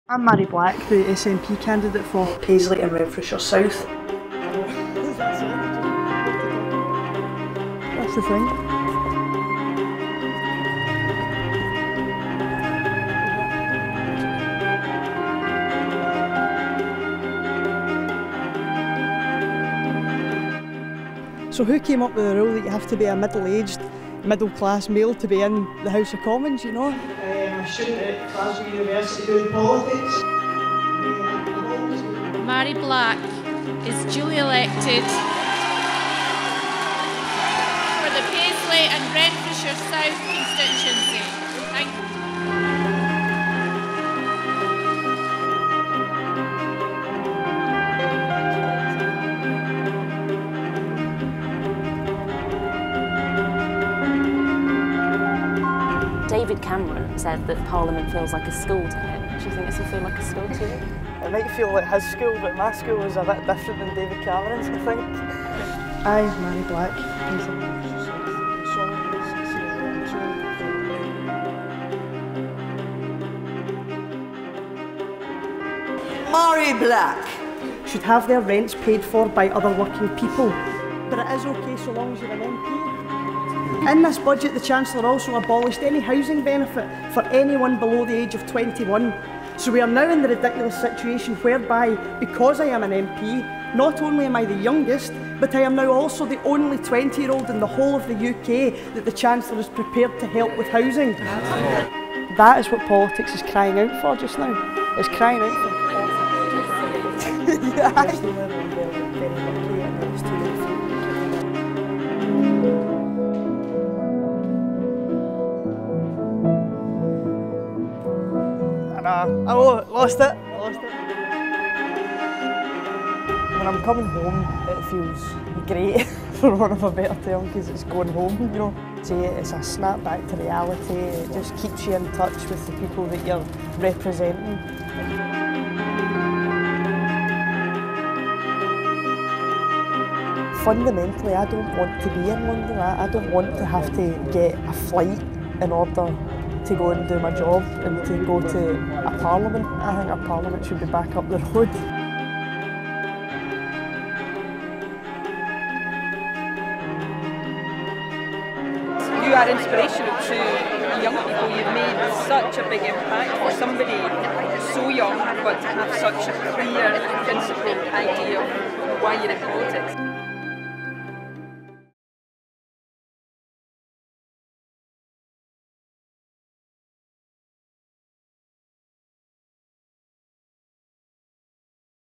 Mhairi Black: Britain's youngest MP
A year in the life of SNP MP Mhairi Black, who was voted into parliament aged just 20.